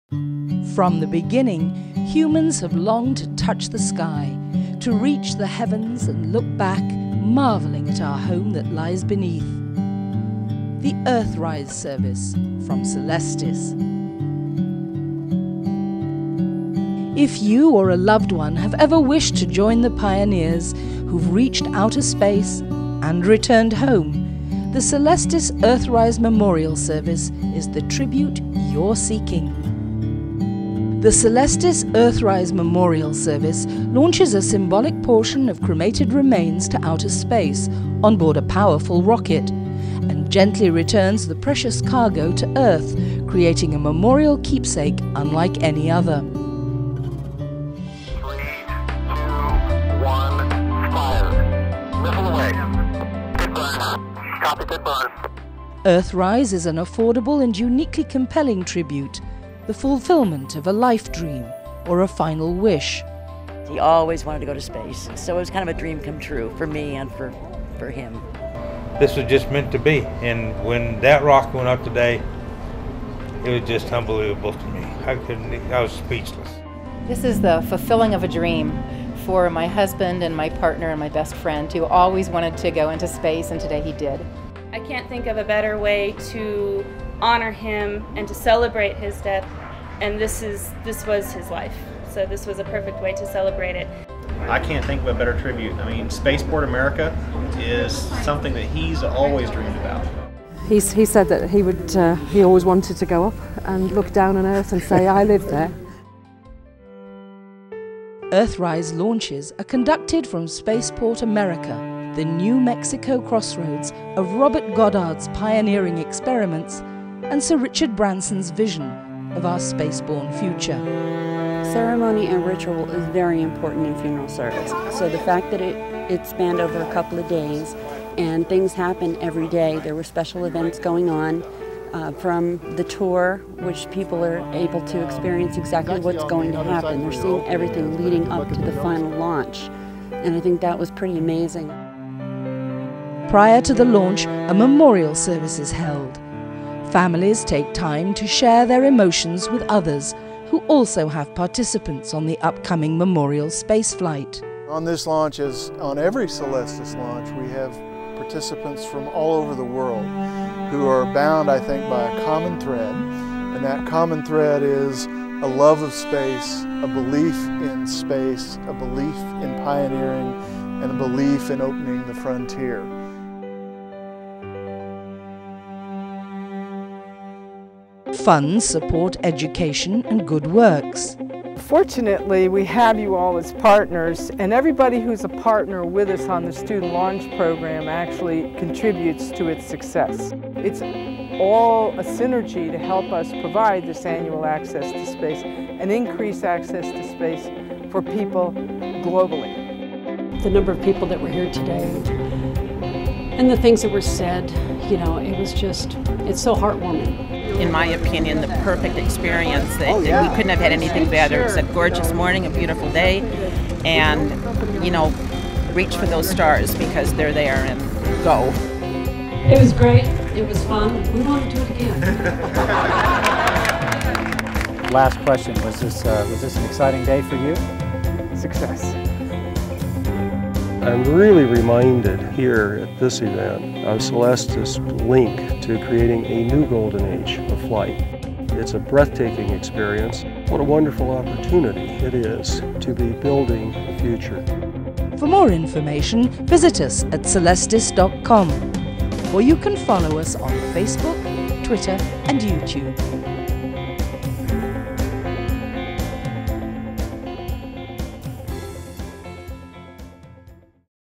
The Earth Rise Memorial Space Flight Service from Celestis
this video provides a comprehensive overview of our Earth Rise Service, available only from Celestis.  Hear testimonials from our participants' family members from all over the world regarding our unique and compelling Memorial Spaceflight.  You may view more full length movies of our service and past launches at our

If you or a loved one have ever considered a space burial where we send a portion of your ashes into space, click below to learn more about our memorial spaceflights.